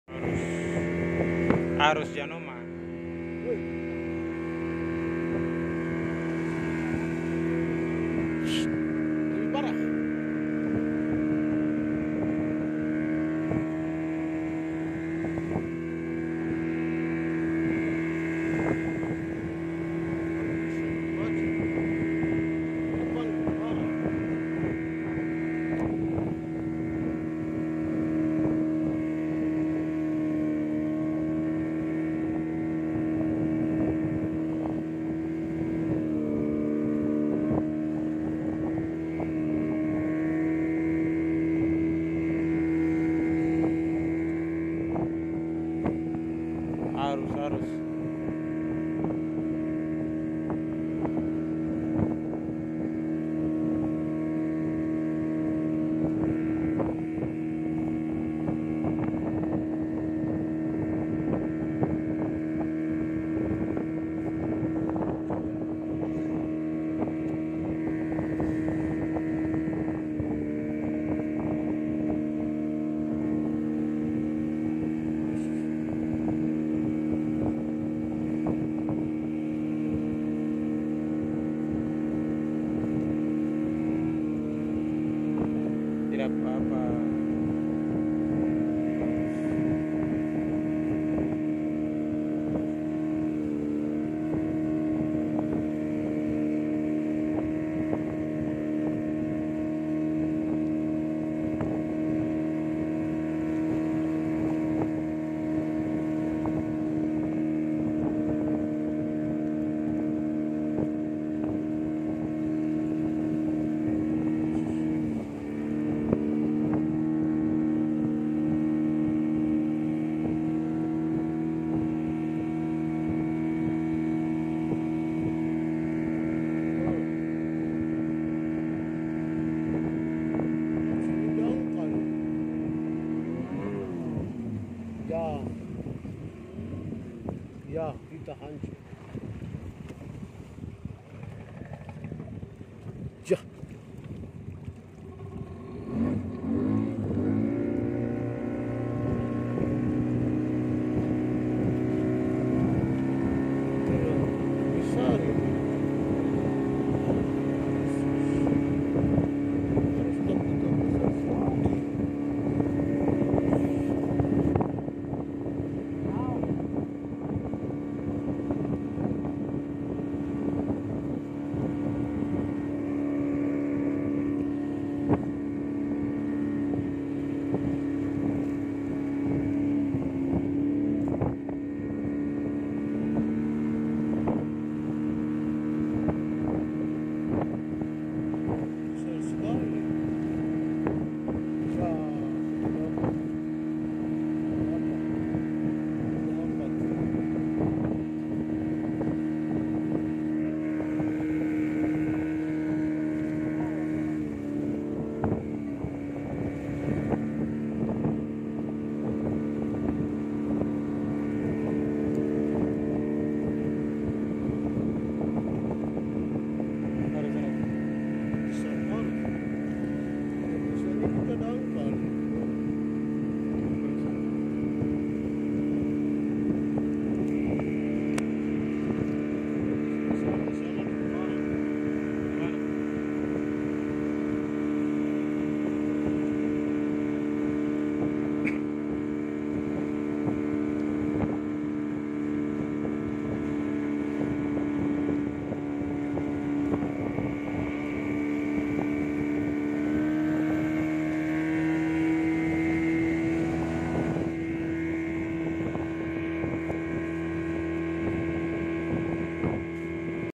Pusaran Arus Terputar Wanoma Distrik Arguni Bawah 🌪️
Pusaran Arus Terputar Wanoma Distrik Arguni Bawah Kabupaten Kaimana Jumat 16 September 2022